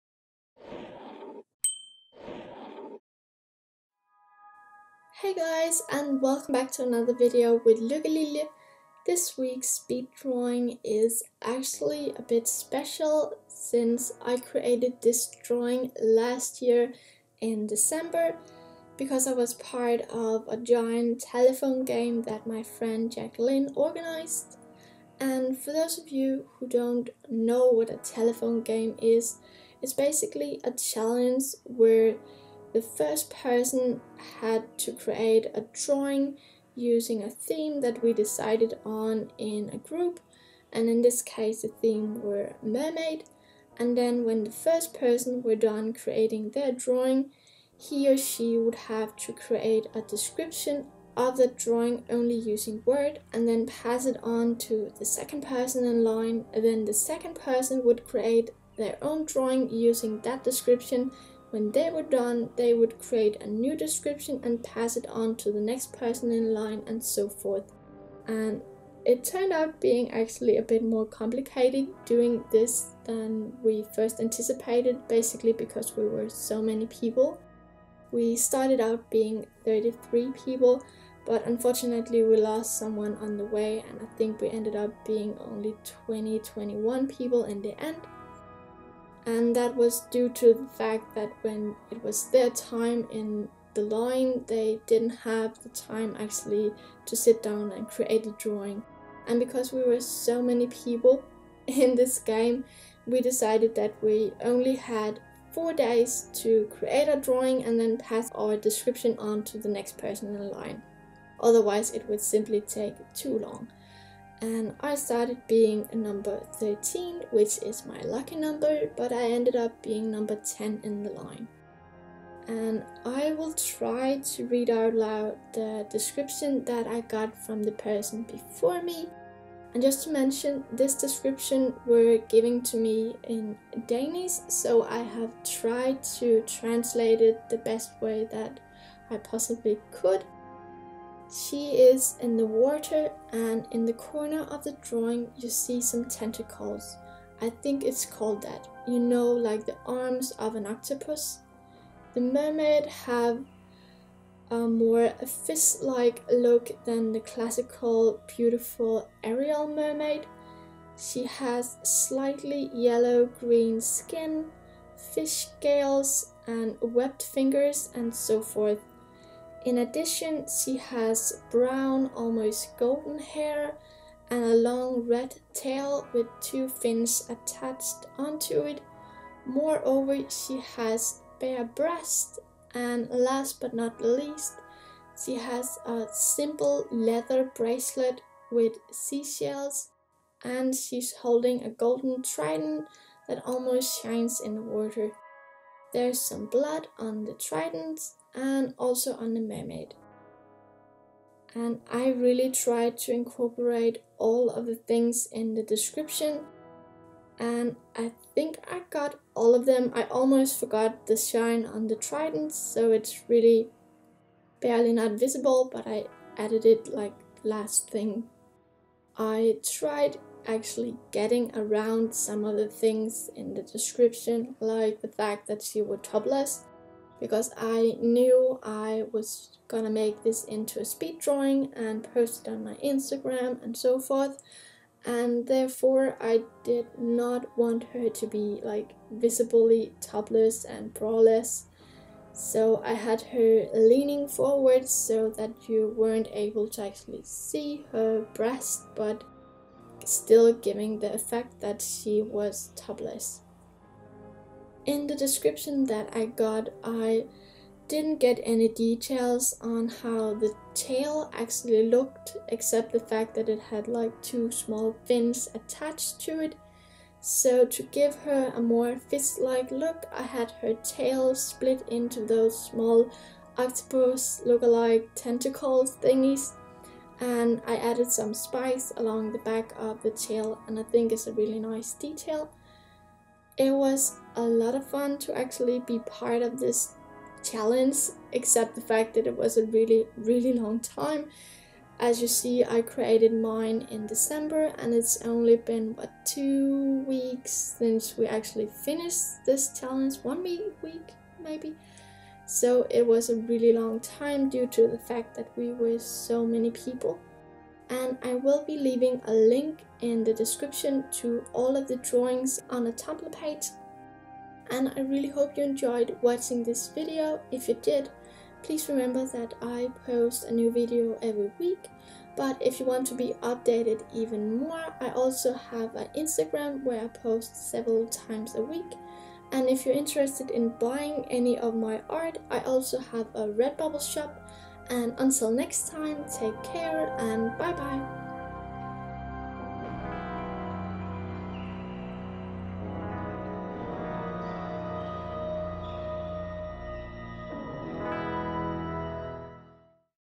GIANT Telephone Game | Drawing Challenge
In the end of 2017 me and 32 others started a giant Telephone Game, and we are finally done, so I can share me contribution with all of you guys.

Program: Photoshop
Tablet: Wacom Intuos pen small
Editing: Premiere Pro
Recording Video and Audio: QuickTime Player